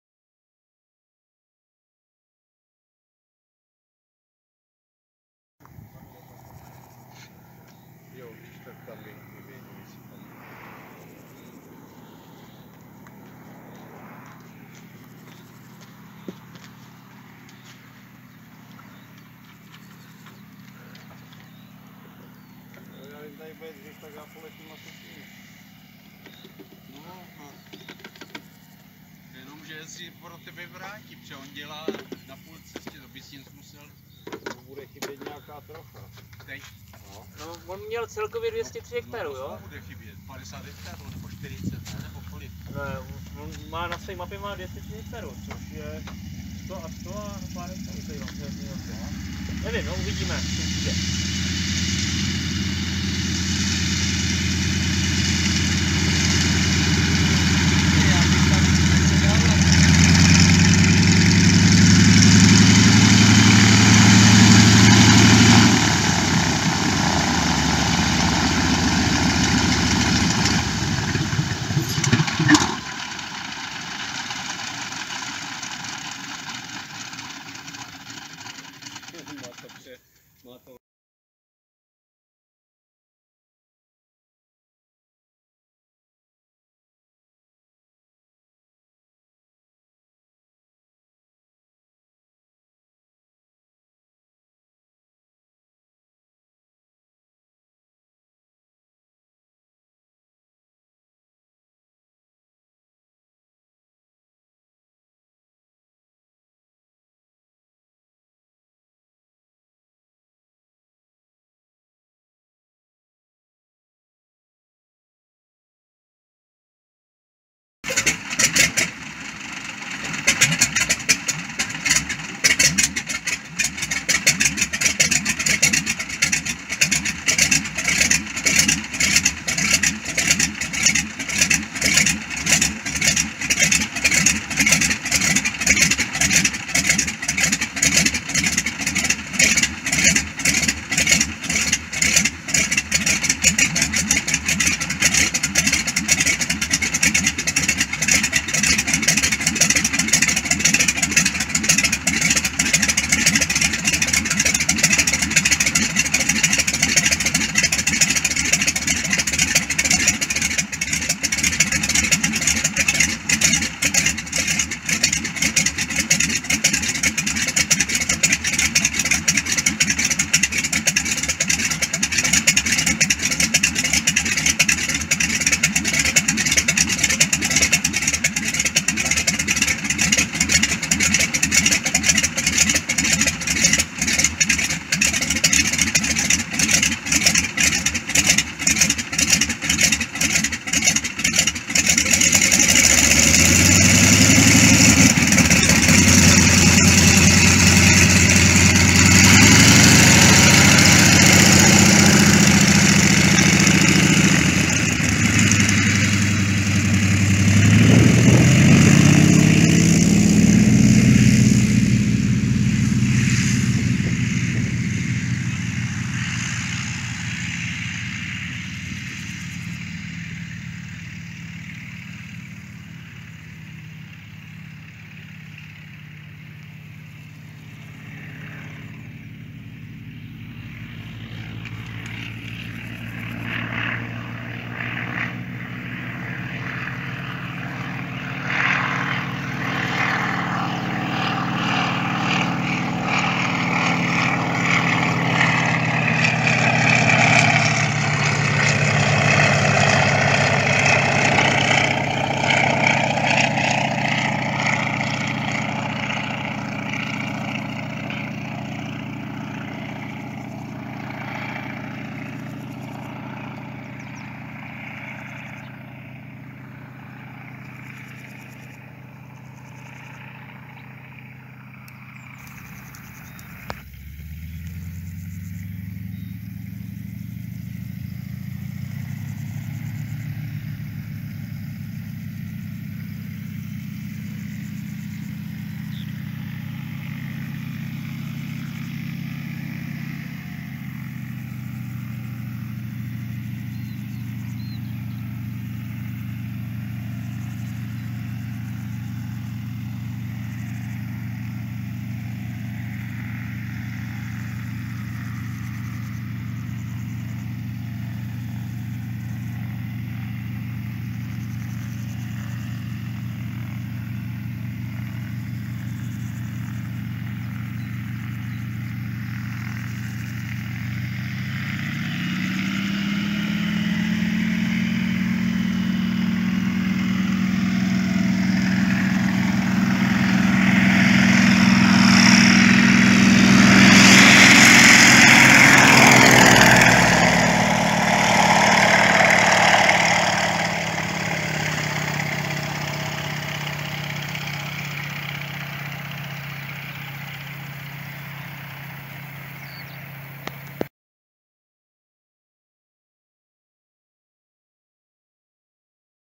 Z 37A Čmelák
Z 37A Čmelák - Přistání a start dvou těchto krásných žluťásků z modelářského letiště u obce Trnávka  .Práškování  pole poblíž obce Trnávka  Jsem velmi rád že tyhle veteránky po dlouhé době viděl v akci